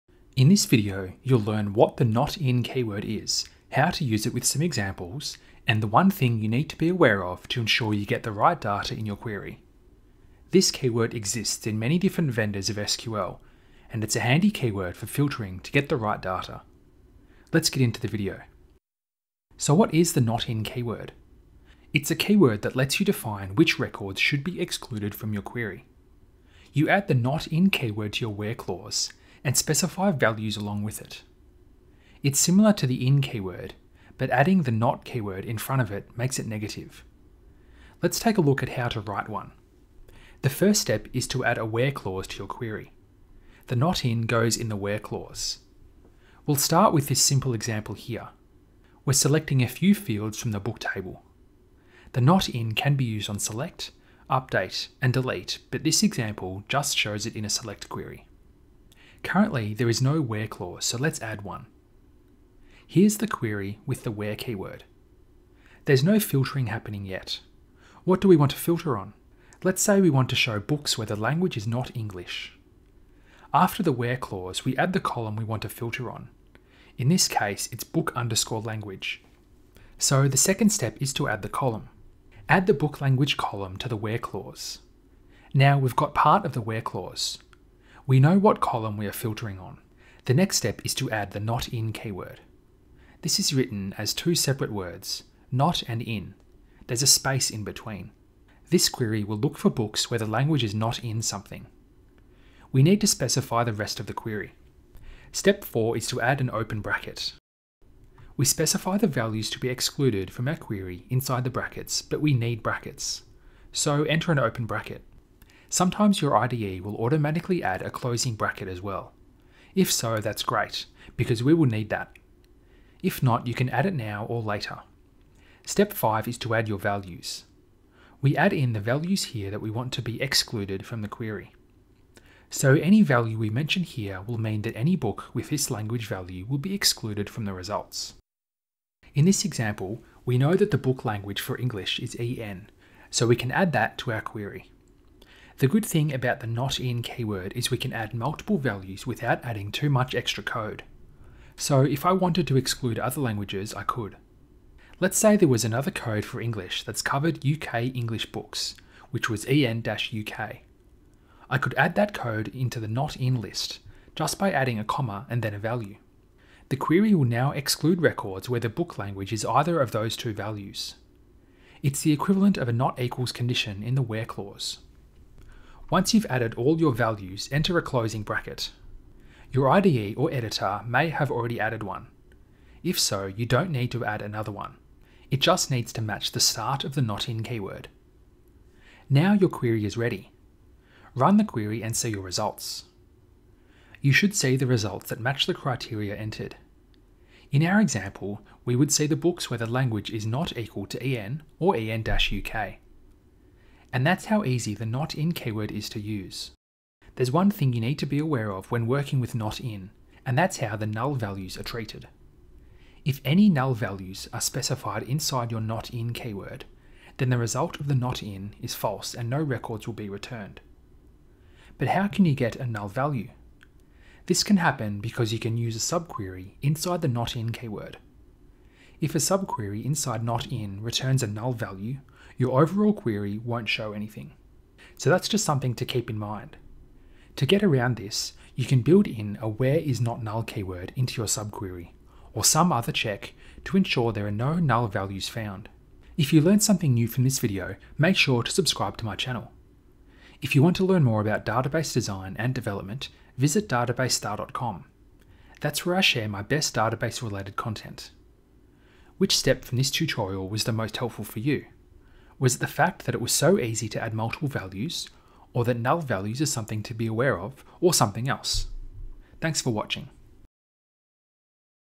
SQL NOT IN Keyword: A Definition and Example
The SQL NOT IN keyword is a useful keyword when working with SQL.

It's based on the IN keyword, which allows you to filter a query based on one or more values. These values can be specified yourself, or you can use a subquery.

The NOT IN keyword is the opposite. It allows you to reverse the comparison, similar to how "not equals" is different to "equals".

If you use NOT IN, and specify values or use a subquery, the query will ignore any records where the column matches any of the values in the NOT IN clause.

It's the same as having a series of WHERE col != value clauses, but it's much easier to type and customise.

In this video, you'll learn what the NOT IN keyword is, and step-by-step instructions on how to write a query that uses the NOT IN keyword.

This video and keyword will work in all major versions of SQL: Oracle, SQL Server, MySQL, PostgreSQL, and others.

There's also one thing to keep in mind, and that's how the NOT IN keyword works with NULL values. I explain more in this video.

Timestamps:
00:00 Start
00:18 What is NOT IN?
00:39 How to add NOT IN with an example
03:36 Beware of NULL